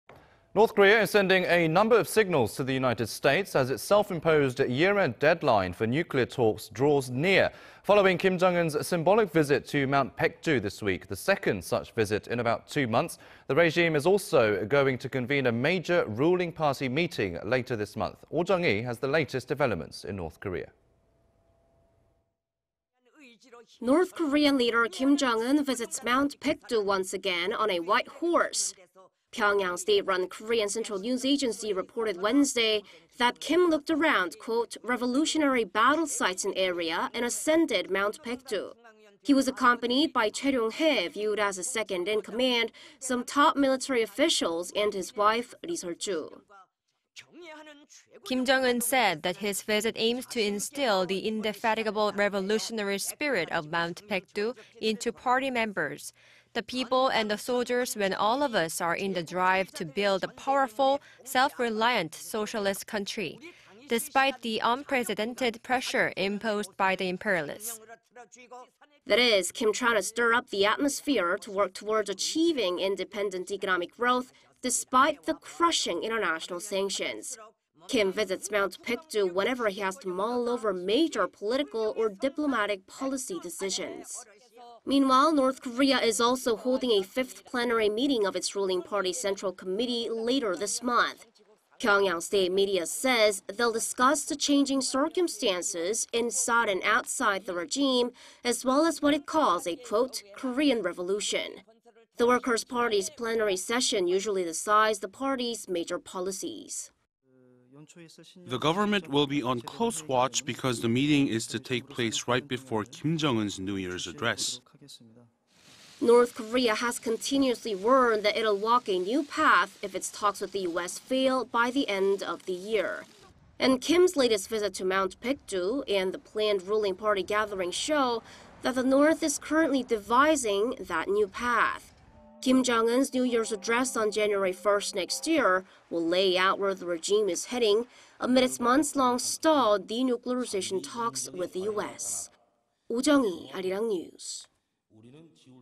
N. Korean leader visits Mt. Baekdu with key aides ahead of year-end deadline in nuke talks
김정은, 연말시한 앞두고 백두산 등정... 北 12월 하순 노동당 전원회의 개최

North Korea is sending a number of signals to the U.S. as its self-imposed year-end deadline for nuclear talks draws near.
Following Kim Jong-un's symbolic visit to Mount Baekdu this week, the second such visit in about two months, the regime is also going to convene a major ruling party meeting later this month.
Oh Jung-hee has the latest developments in North Korea.
North Korean leader Kim Jong-un visits Mount Baekdu once again on a white horse.
Pyeongyang's state-run Korean Central News Agency reported Wednesday that Kim looked around "revolutionary battle sites" in area and ascended Mount Baekdu.
He was accompanied by Choe Ryong-hae, viewed as his second-in-command, some top military officials, and his wife Ri Sol-ju.
"Kim Jong-un said that his visit aims to instill the indefatigable revolutionary spirit of Mount Baekdu into party members, the people and the soldiers when all of us are in the drive to build a powerful, self-reliant socialist country, despite the unprecedented pressure imposed by the imperialists."
That is Kim trying to stir up the atmosphere to work towards achieving independent economic growth despite the crushing international sanctions.
Kim visits Mount Baekdu whenever he has to mull over major political or diplomatic policy decisions.
Meanwhile, North Korea is also holding a fifth plenary meeting of its ruling party's central committee later this month.
Pyeongyang's state media says... they'll discuss the changing circumstances inside and outside the regime... as well as what it calls a "Korean Revolution."
The Workers' Party's plenary session usually decides the party's major policies.
"The government will be on close watch because the meeting is to take place right before Kim Jong-un's New Year's Address."
North Korea has continuously warned that it will walk a "new path" if its talks with the U.S. fail by the end of the year.
And Kim's latest visit to Mount Baekdu... and the planned ruling party gathering show that the North is currently devising that "new path."
Kim Jong-un's New Year's Address on January 1st next year will lay out where the regime is heading amid its months-long stalled denuclearization talks with the U.S.